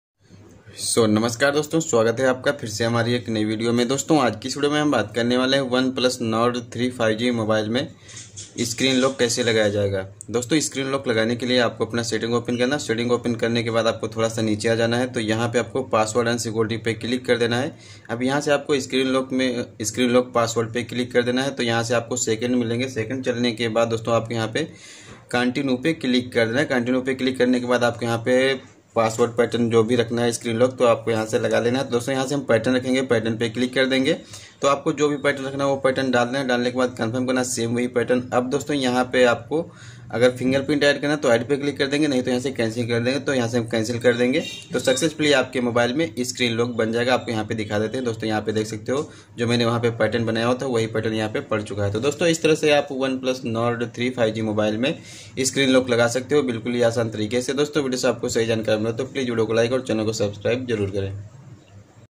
OnePlus Nord 3 5g me screen lock kaise lagaye | how to set screen lock OnePlus Nord 3 5g
hello friends Aaj ki is video ke madhyam se maine aap logon ko bataya hai ki OnePlus Nord 3 5g mobile me screen lock kaise lagaye